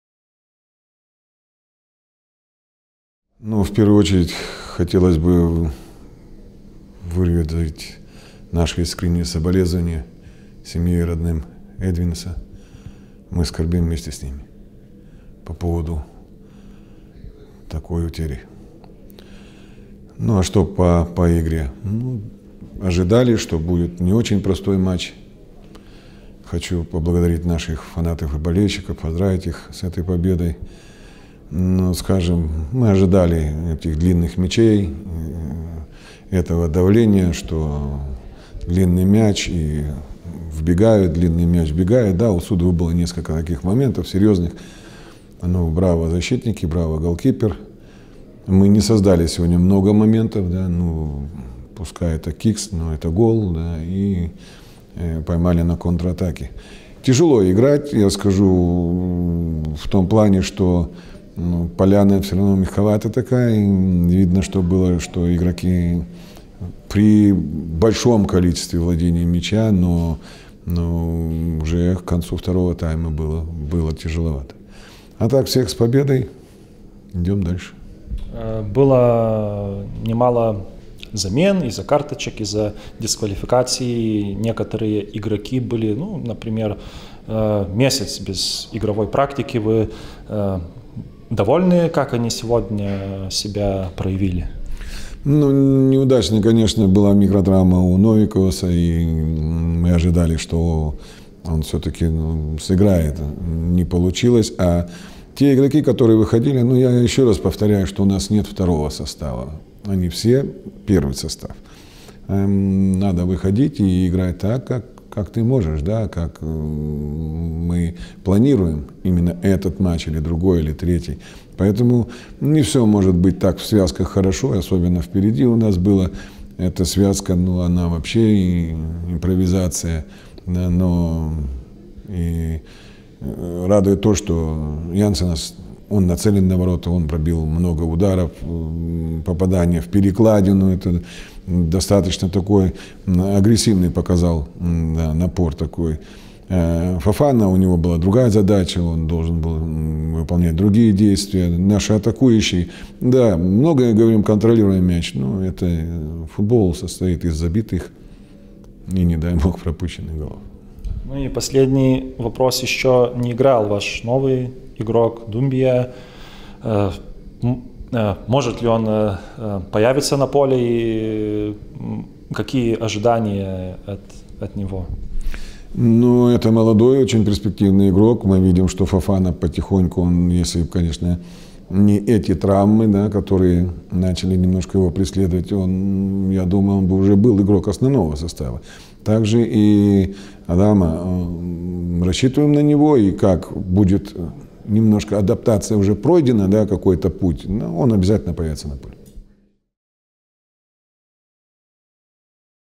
Čeburin | Trenerio komentaras | Interview | Sūduva - Žalgiris [CC]
Vladimir Čeburin komentaras po rungtynių Sūduva - Žalgiris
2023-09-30

Rusų kalba, titrai lietuviškai